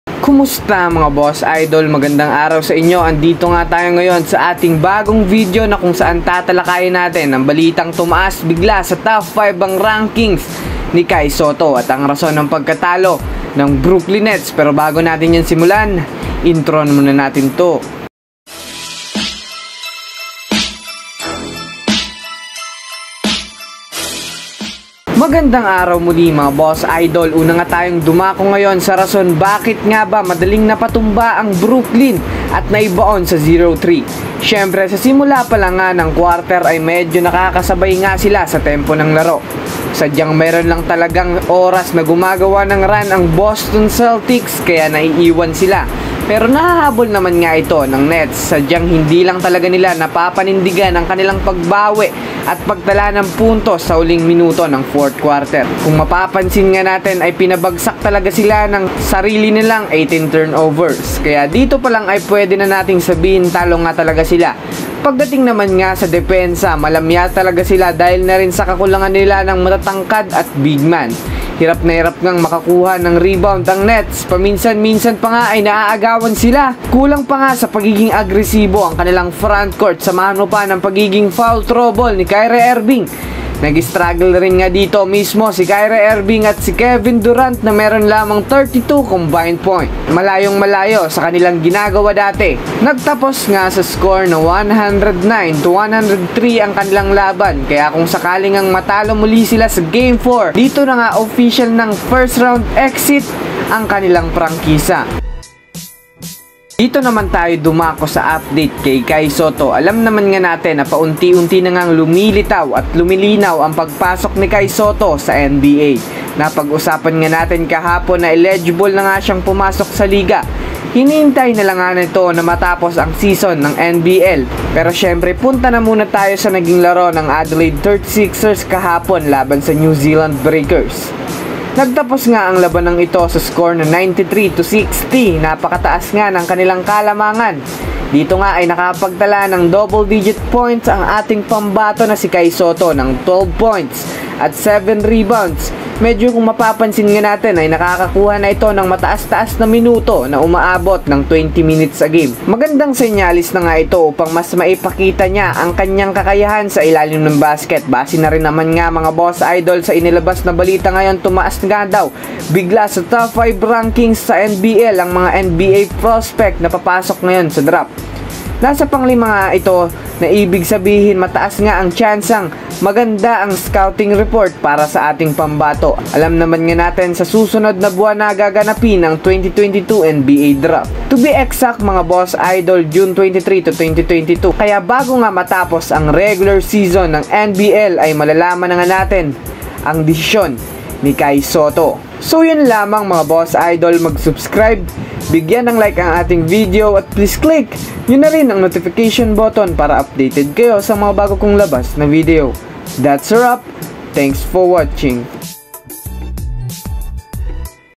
Kai SOTTO TUMAAS BIGLA sa TOP 5 | RASON ng PAGKATALO ng NETS
Halina't pag usapan natin ngayon ang balitang tumaas nga bigla si Kai Sotto at ang rason ng pagkatalo ng Nets.